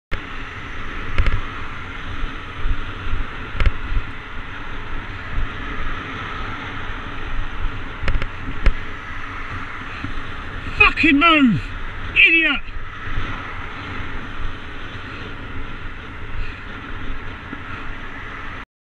EY14 XFL Dangerously Close Pass
EY14 XFL Dangerously Close Pass. Sorry for the swearing & shouting but this really did take me by surprise.

EY14 XFL
EY14XFL